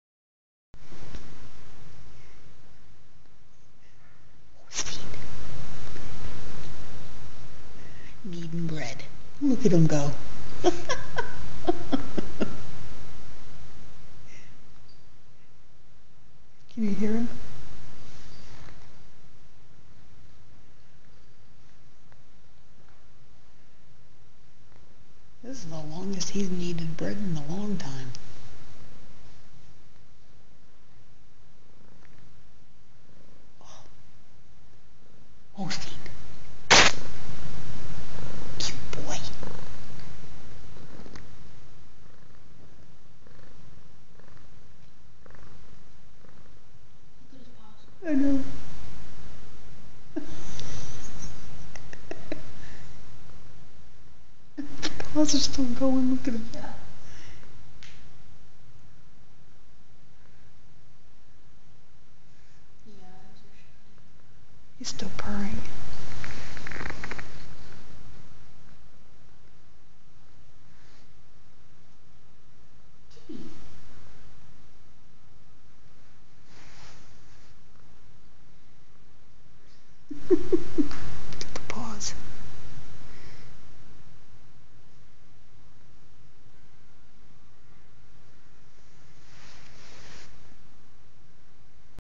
Holstein "Kneading Bread" on his favorite blanket. You can even hear him purring!